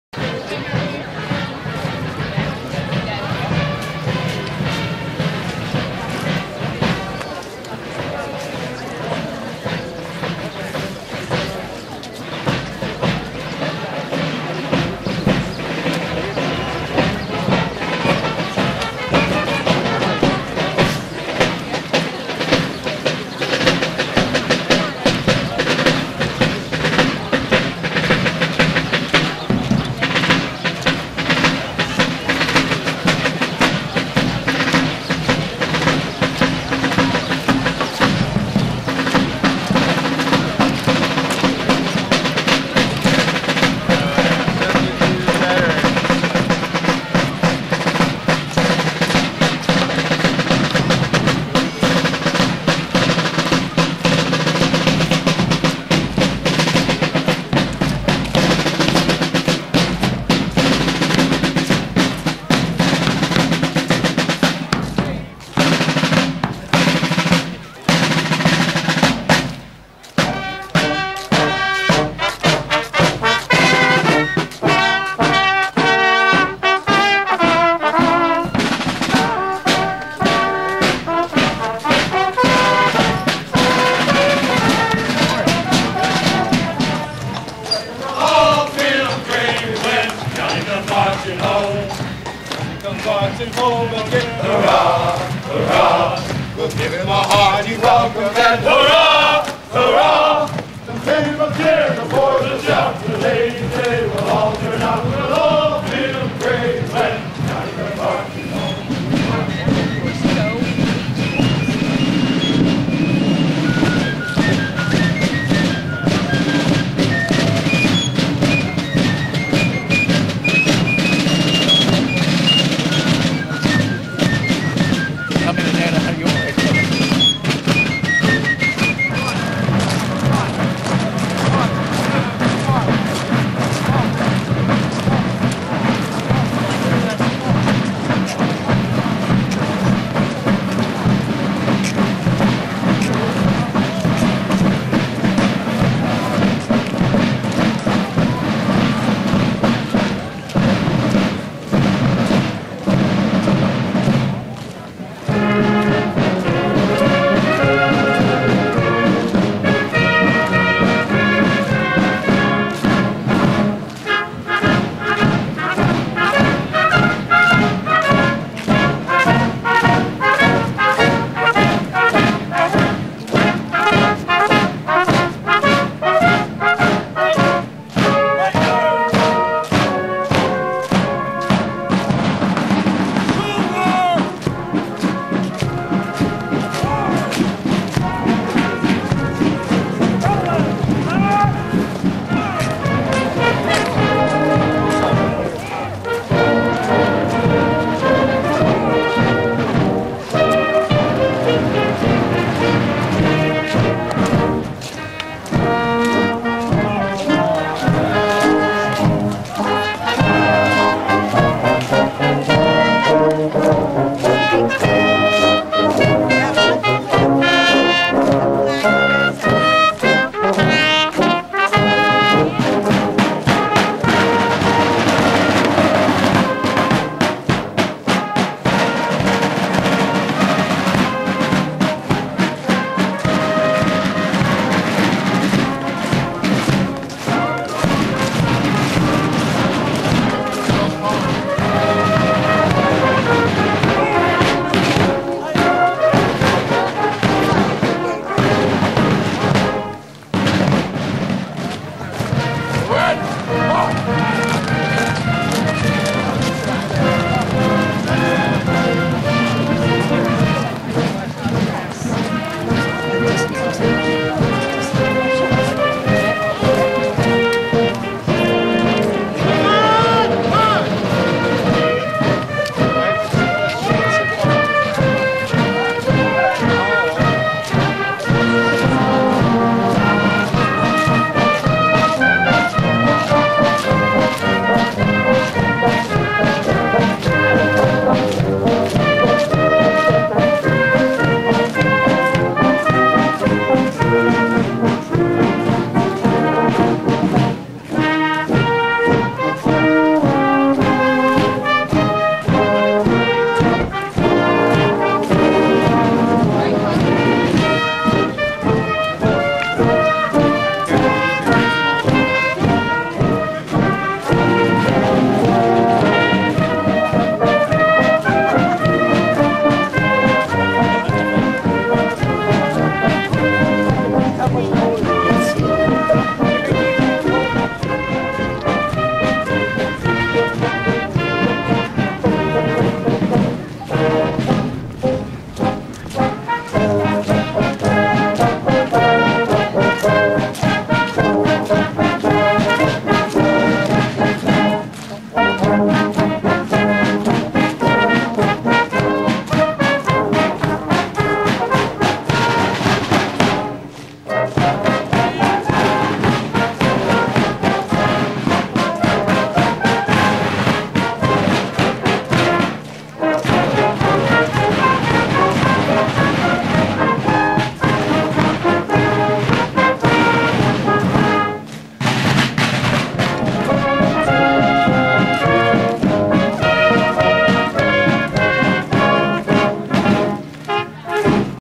Brass Bands at Remembrance Day Gettysburg PA 2007
Brass Bands at Remembrance Day 2007 in Gettysburg PA
The Federal City Brass Band